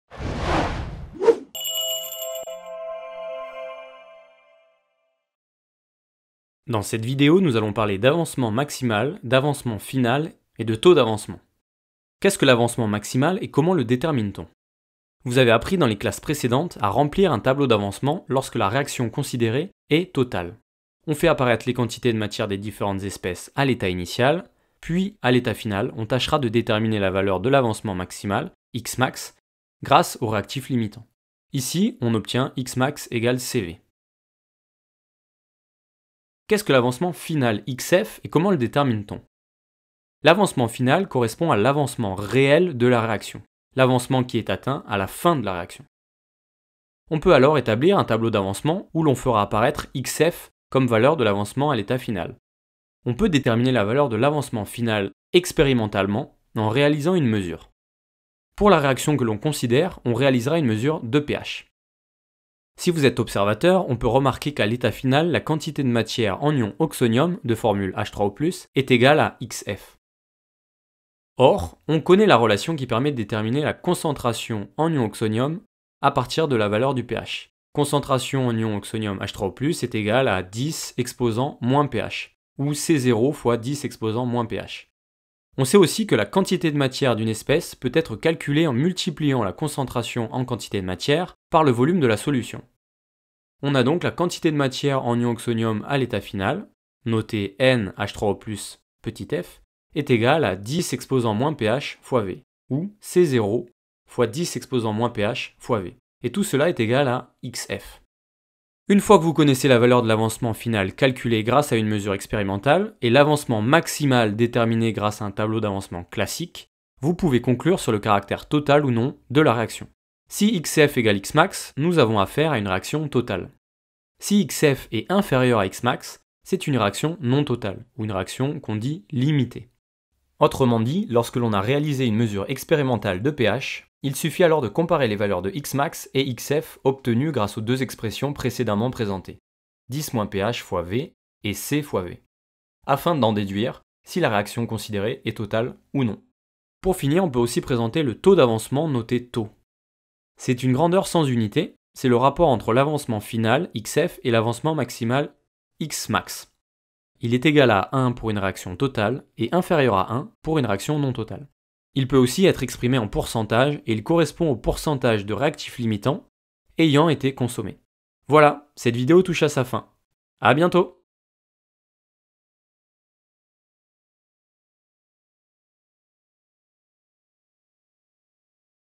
xmax, xf et tau ? Qu'est-ce que l'avancement maximal, l'avancement final et le taux d'avancement ?
Dans cette vidéo, nous parlerons de l'avancement maximal xmax, l'avancement final xf et le taux d'avancement final.